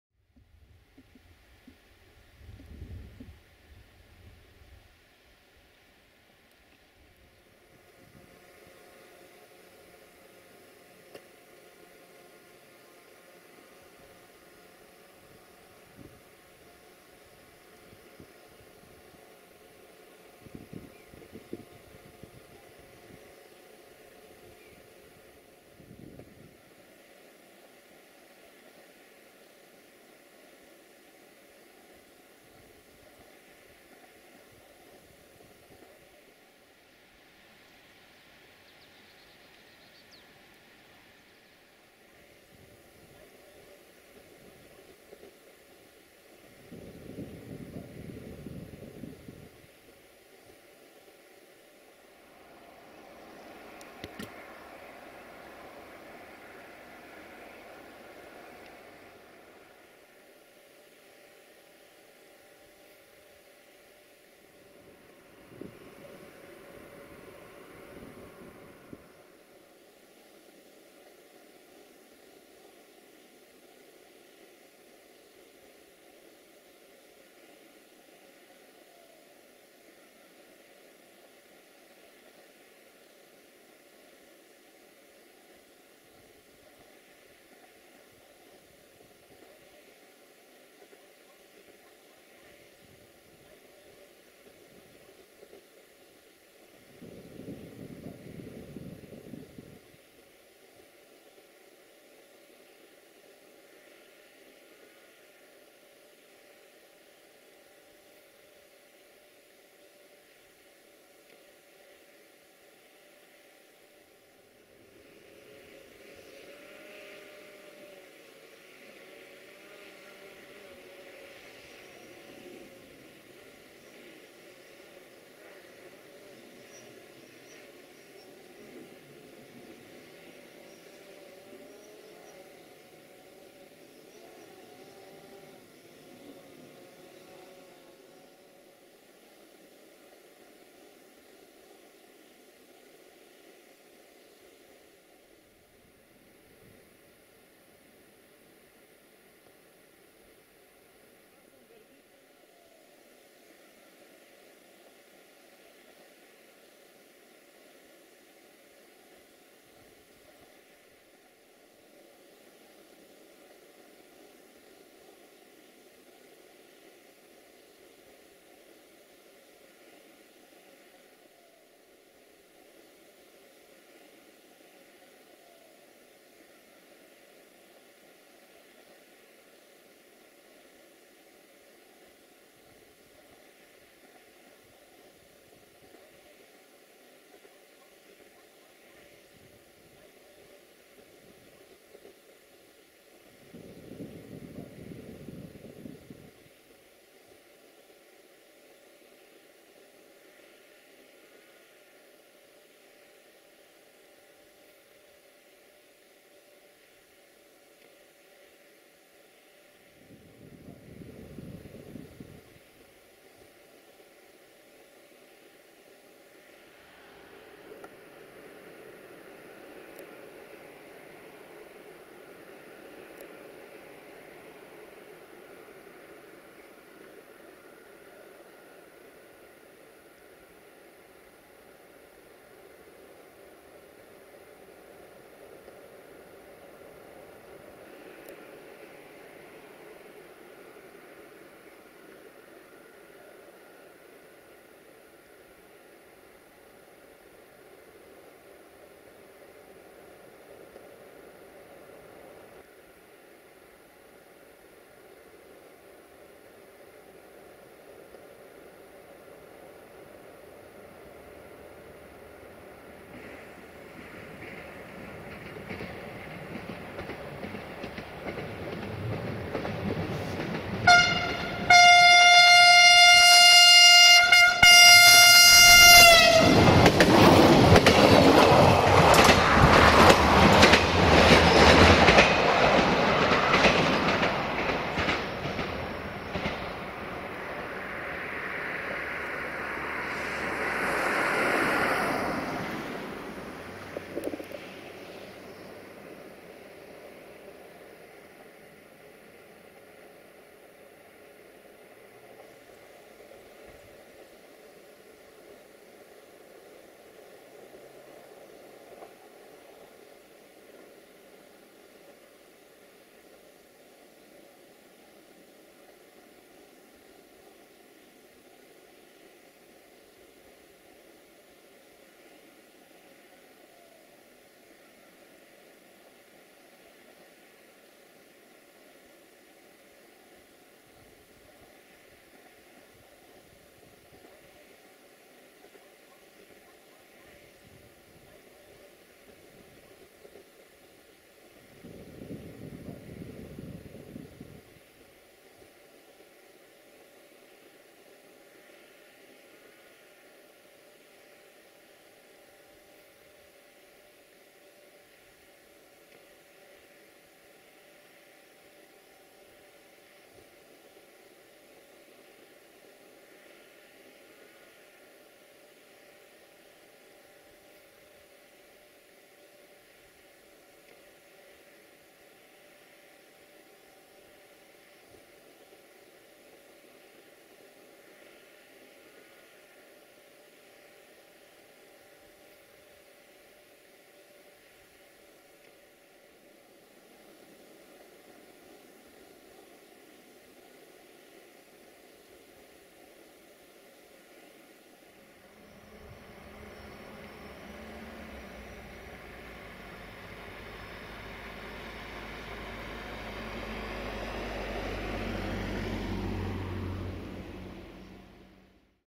[Drone]IR1533 Cluj Napoca-Oradea in Defileul Crisului Repede Canyon [Autumn Edition]19 November 2017
1:IR1533 - Cluj Napoca - Oradea - Automotor Sageata-Albastra~Desiro VT642 DMU RO-SNTFC

[Autumn Edition]

19 November 2017